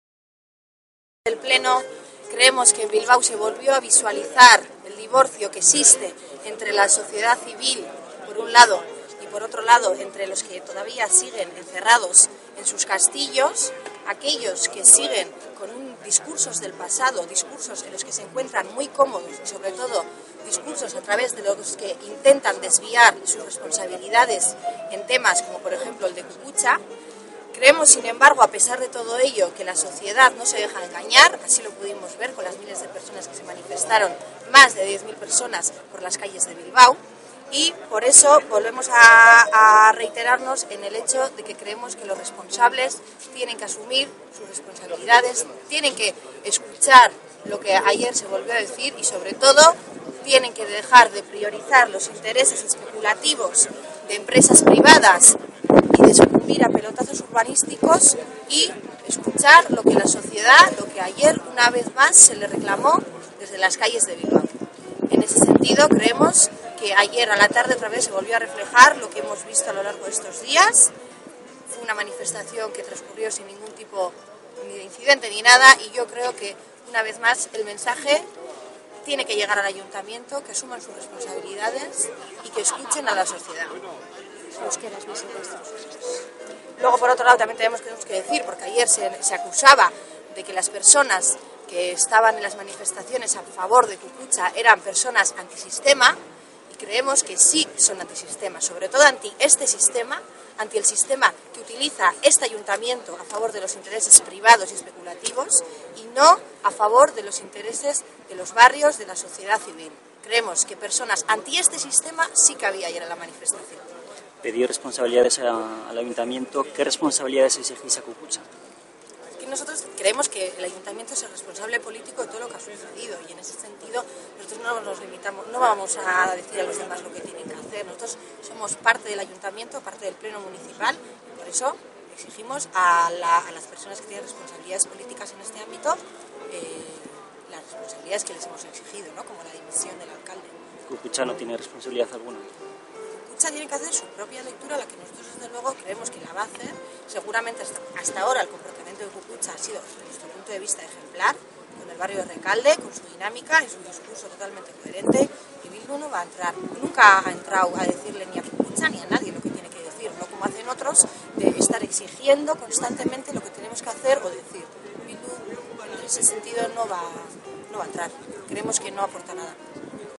Bildu considera que existe un divorcio entre la sociedad y la clase política (Kukutza)
La concejala de Bildu valorá la discusión en el pleno de la moción presentada por este grupo sobre el desalojo y derribo del Gaztetxe Kukutza III . Aitziber Ibaibarriaga considera que la manifestacióna  favor de Kukutza evidencia un divorcio entre la sociedad y los políticos que viven encerrados en sus castillos.